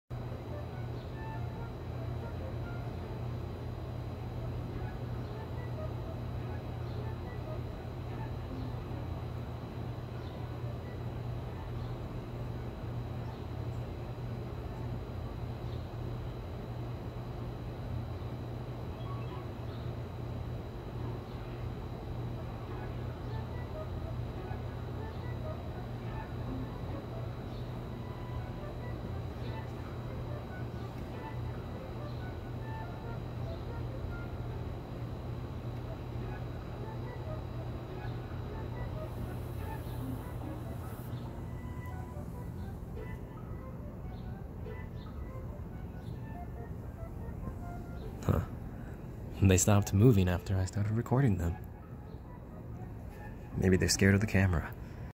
Wasps chilling with OOcoo
Took this video a month ago?
5 wasp and one queen. I have watched this nest for 4 months or so, maybe 5 now. They haven't grown too much, maybe because of the small space she nested in. There's a new nest outside at the end of the balcony that's already almost twice as large.